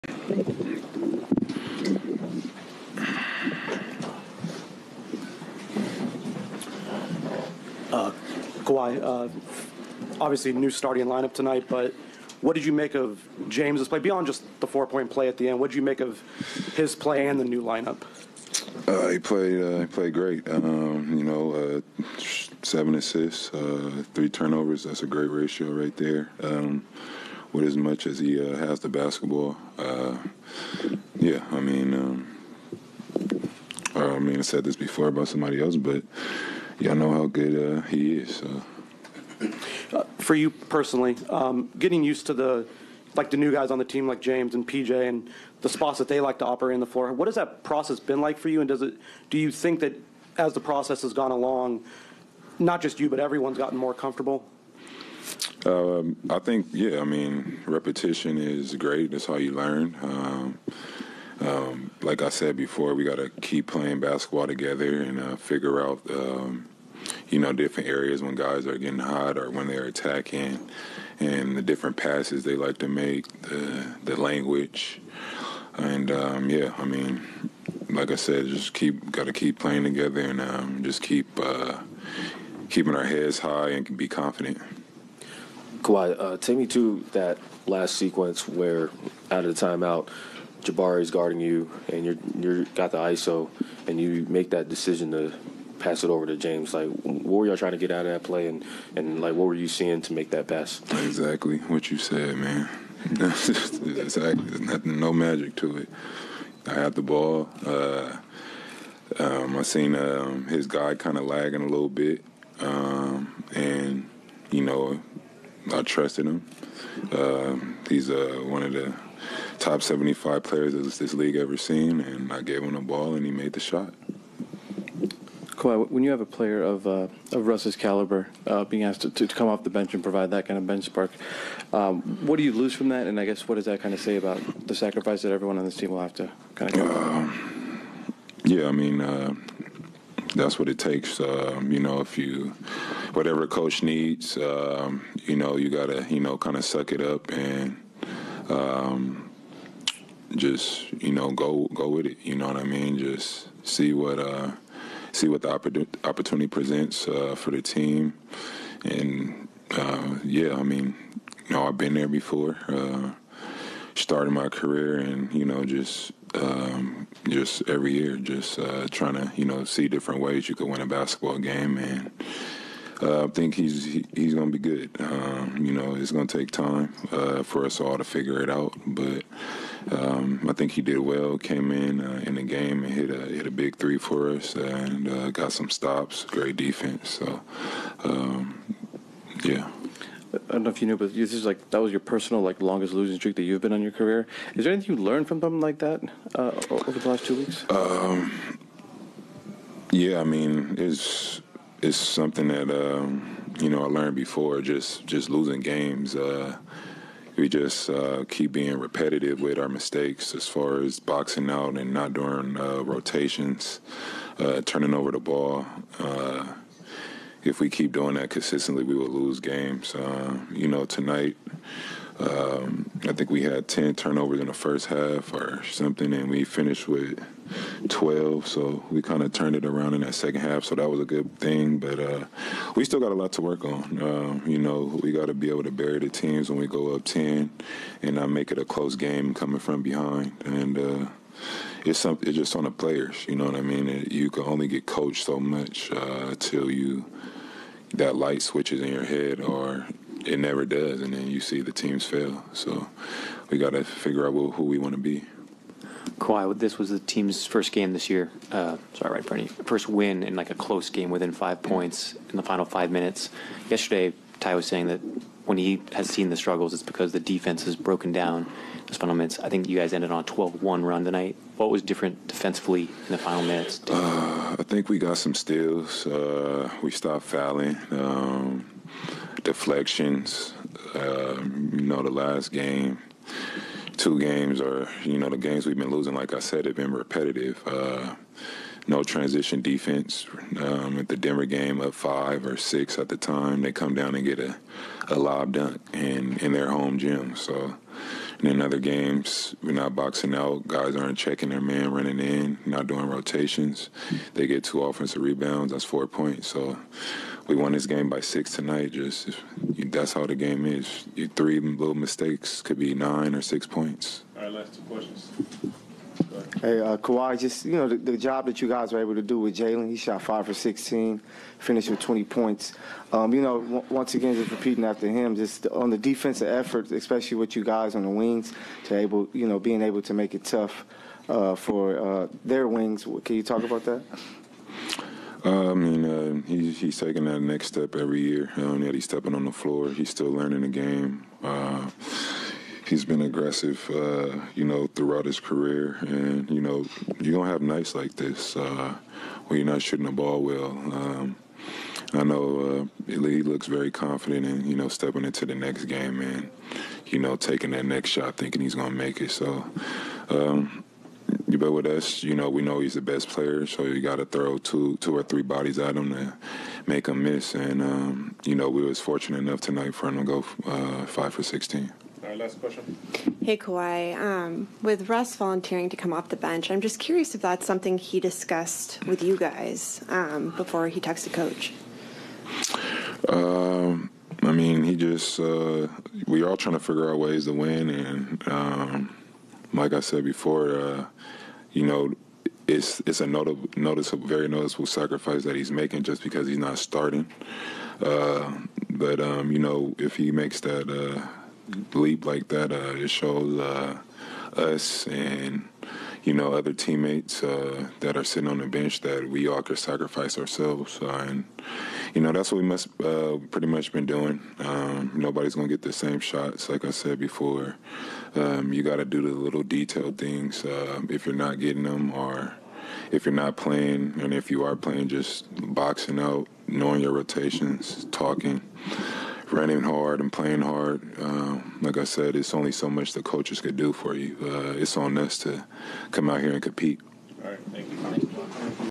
James Harden hits game winner; Kawhi Leonard calls jamesharden 1 of best ever during postgame
"He's one of the top 75 players the league has ever seen. I gave him the ball and he made the shot."

Kawhi on James Harden's game-winning 4-point play. Harden finished with 24 PTS (73% FG), 9 REB (game high) & 7 AST (team high) vs his former team.